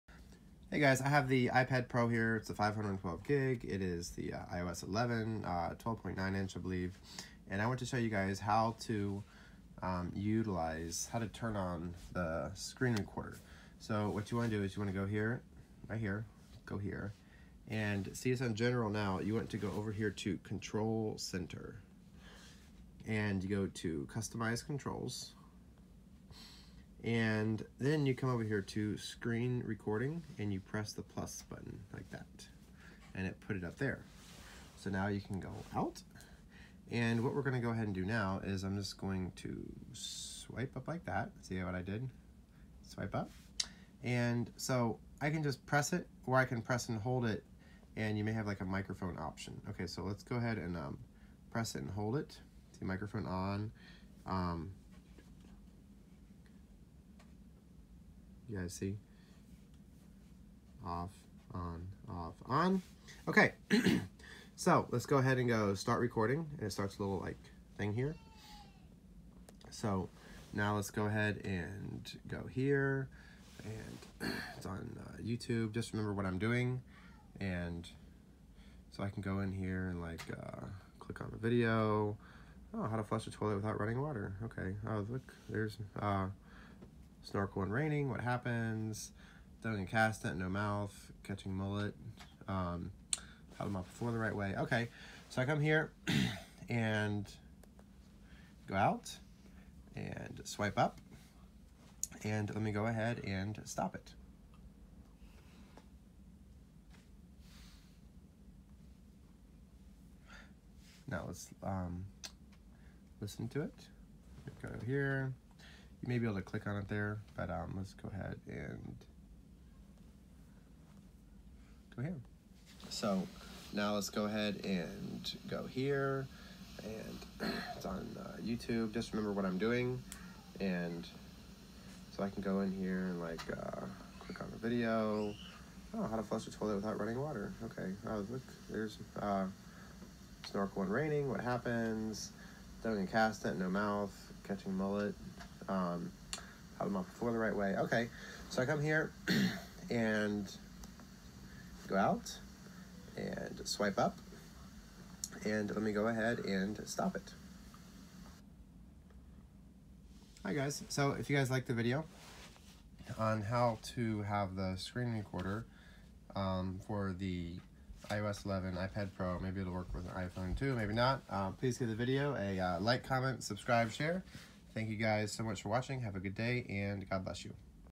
How to screen record apple iPad Pro
How to screen record apple iPad Pro tablet
Earbuds with mic
screen recording is a really cool thing for gamers and it’s nice to know how to do. The apple iPad Pro tablet can be a great option for gaming and streaming gameplay on YouTube.